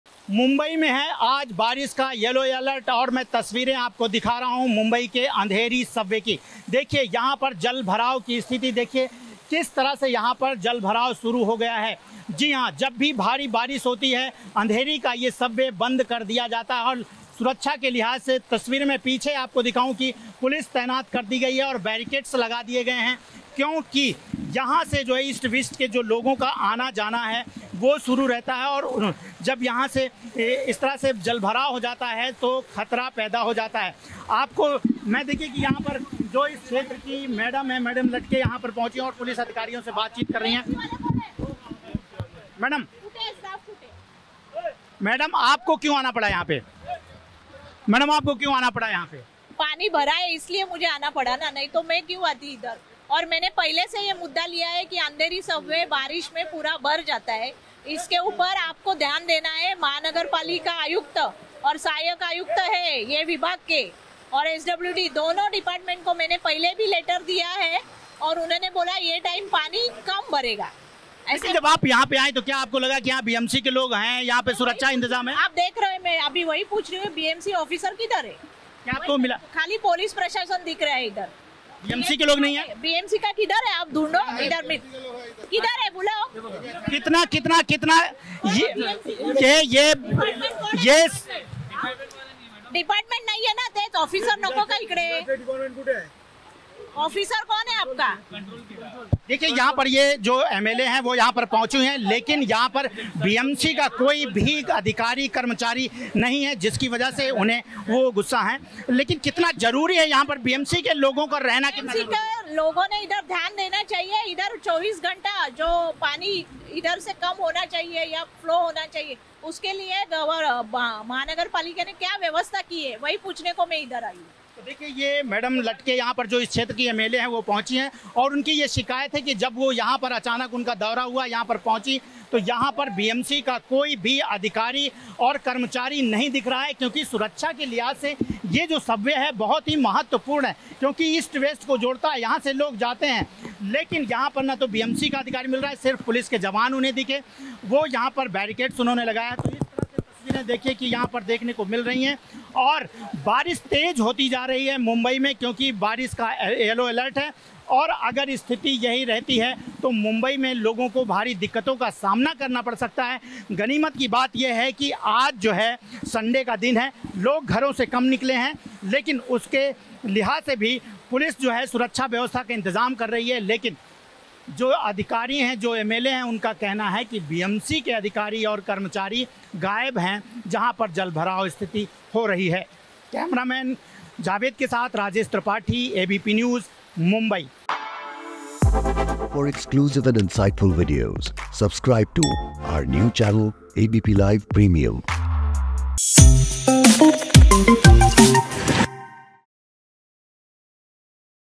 Mumbai में आज भारी बारिश के बीच येलो अलर्ट जारी, मौके पर जायजा लेने पहुंची अंधेरी क्षेत्र की विधायक
मुंबई में आज है बारिश का येलो अलर्ट . मुंबई में हो रही तेज बारिश की वजह से l अंधेरी सबवे में पानी भरा. सुरक्षा के लिहाज से पुलिस ने सबवे को बंद किया. मौके पर जल भराव का जायजा लेने पहुंची अंधेरी क्षेत्र की विधायक ऋतुजा लटके. उनका आरोप है कि यहां पर बीएमसी के कोई भी अधिकारी मौजूद नहीं है जबकि यहां पर सुरक्षा के लिहाज से बीएमसी के अधिकारियों को होना चाहिए क्योंकि अंधेरी के ईस्ट वेस्ट को जोड़ने वाला एकमात्र रास्ता है और यहां पर जल भराव की वजह से लोगों के सामने भारी दिक्कत खड़ी हो जाती है।